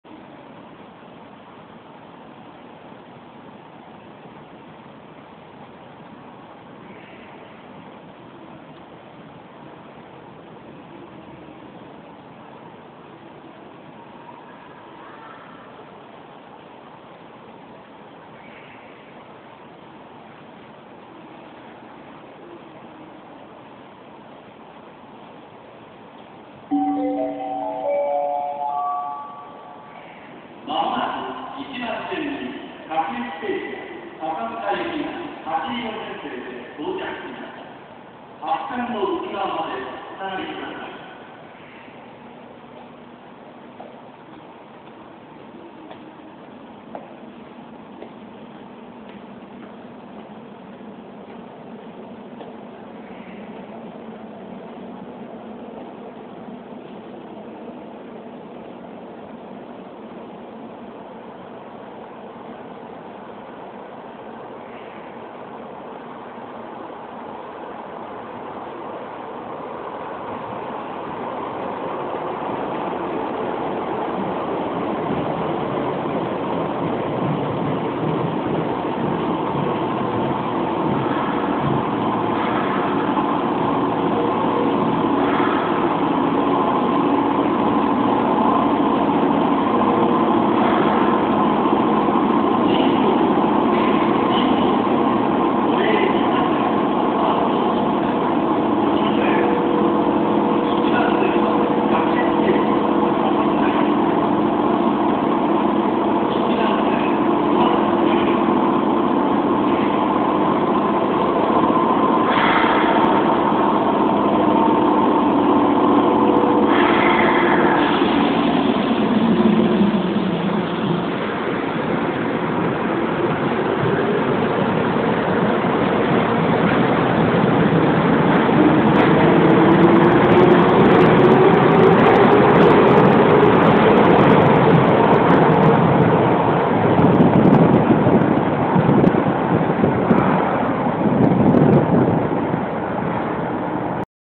都営地下鉄神保町駅 1番線電光掲示板＆駅自動放送 接近＆発車
都営新宿線神保町駅1番線の電光掲示板と各停笹塚行き接近放送と発車放送です。
村山明さんの放送です。

スピーカー：TOA
路線：新宿線 新宿・京王線方面